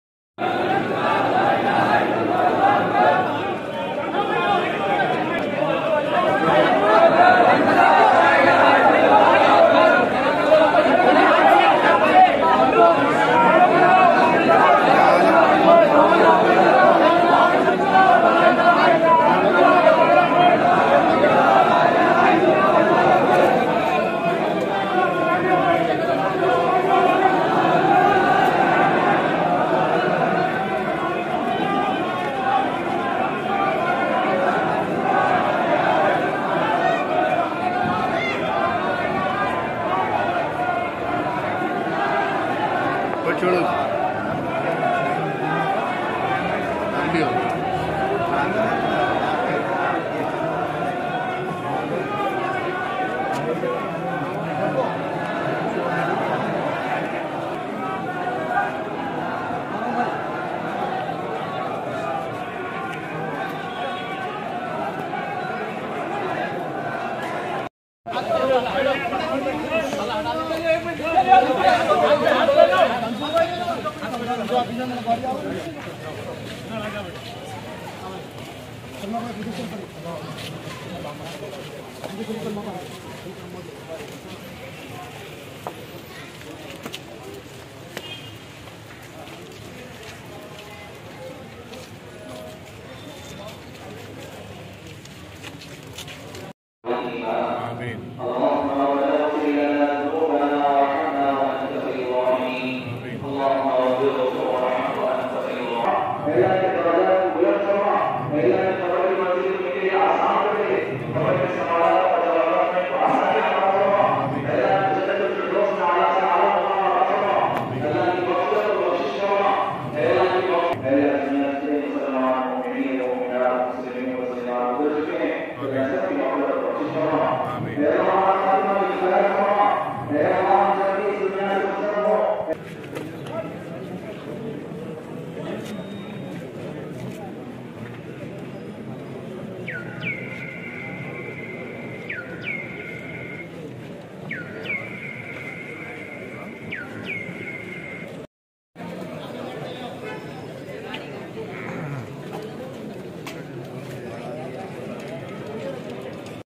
AIMIM Ke Haroon Khan Sab Ki Tadfeen Akber Owaisi Aur Hazaroon LooGaoon Ne Ki ShirKat || HYDTAK
Aimim Ke Haroon_Khan_Sab Ki Tadfeen ...
Akber Owaisi Aur Hazaroon LooGaoon Ne Ki ShirKat .